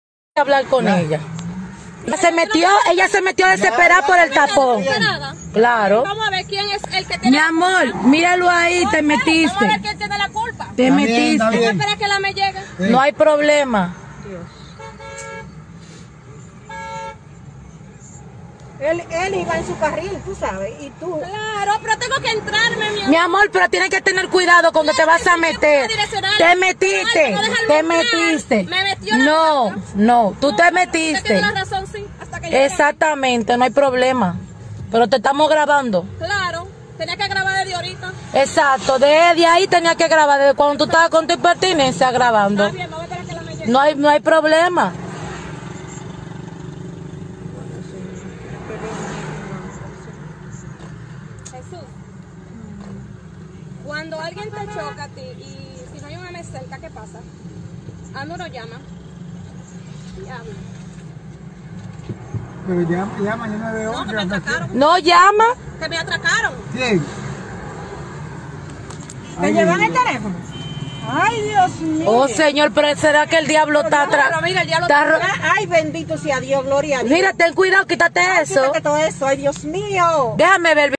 La atracaron mientras intentaba llamar en medio de un accidente.
Un elemento atraco a una mujer cuando está discutía con otras personas tras ocacionarse un accidente de tránsito.

En el vídeo se observa la mujer cuando intenta llamar y le arrbatan el celular por detrás.